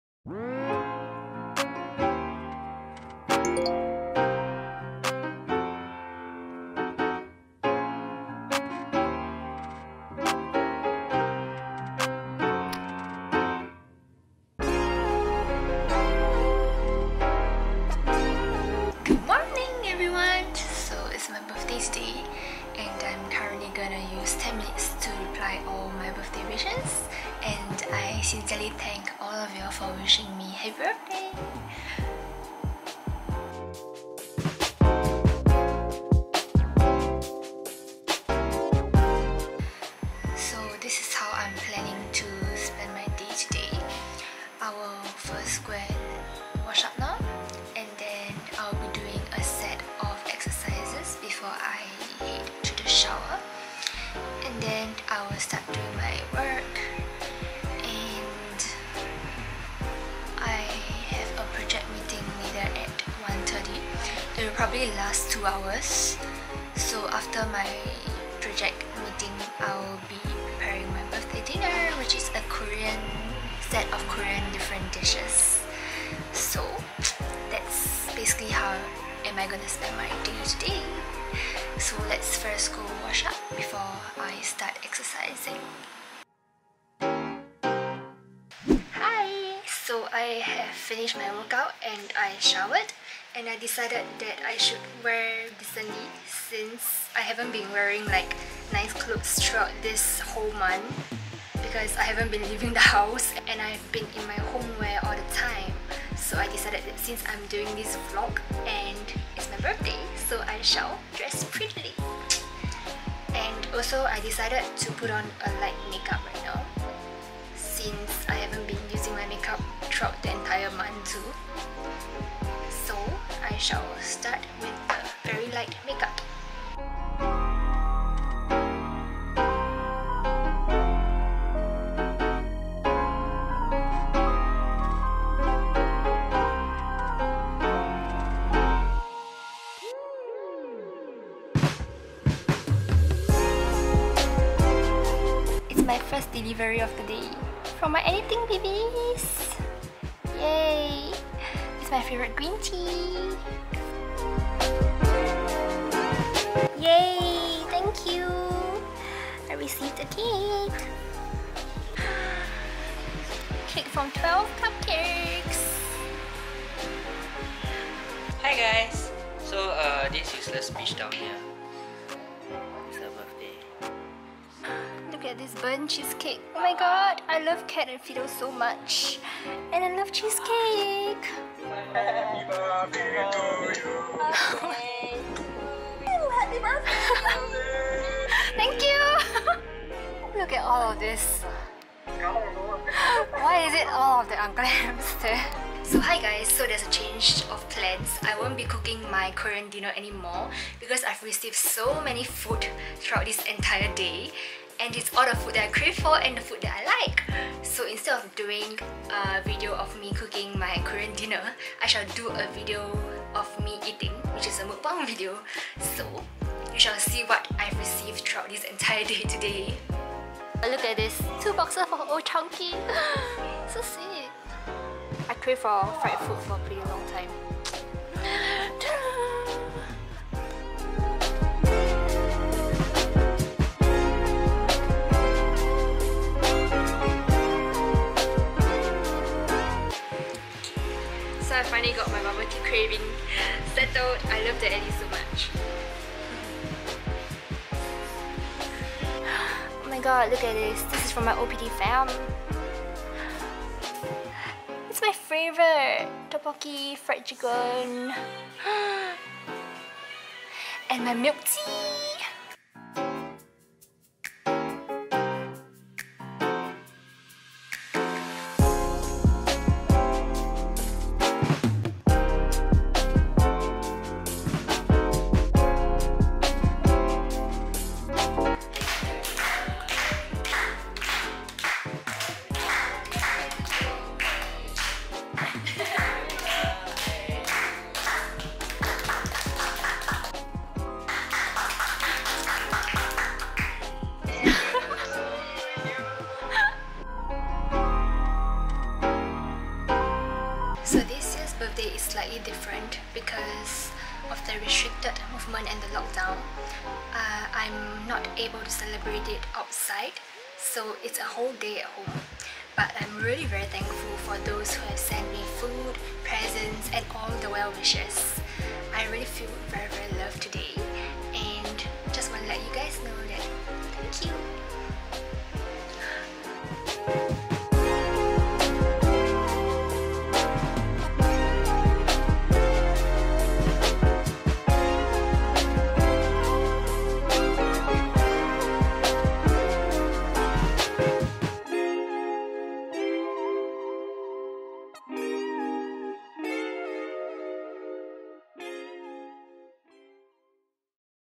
VLOG #2 | Such a unique birthday experience! 🎂
I turned 22! So it was a unique birthday experience this year because of the circuit breaker, I couldn't go out to celebrate my birthday. However, it was still fun because my friends ordered so much food for me 😍 I did this vlog with the intention to prepare my birthday dinner at home by cooking, but I received 7 birthday cakes and there were so many food deliveries, the food coming didn't stop the entire day! So I had to cancel my cooking plans for dinner, but I am really very thankful to those who have sent me food & all the well-wishes! It was indeed a unique birthday experience! :D